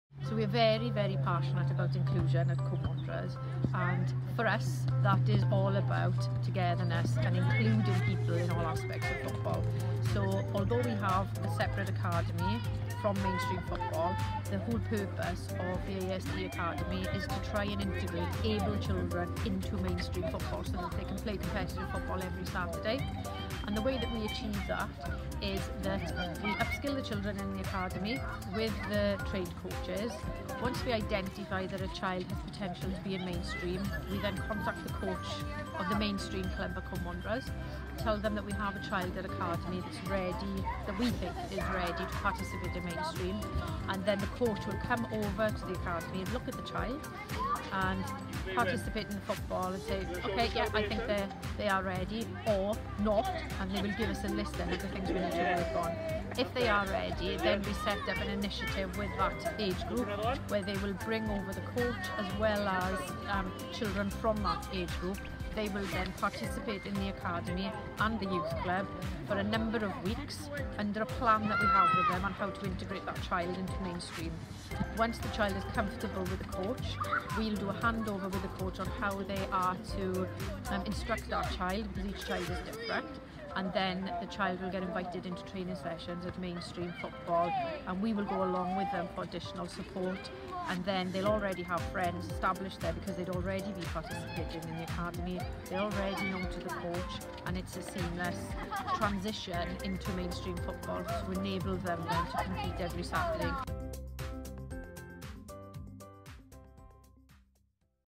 Cwm Wanderers - Importance of Inclusion
Here Andrea explains why it's so important to everybody at Cwm Wanderers to be inclusive and what they do to make the club as inclusive as possible.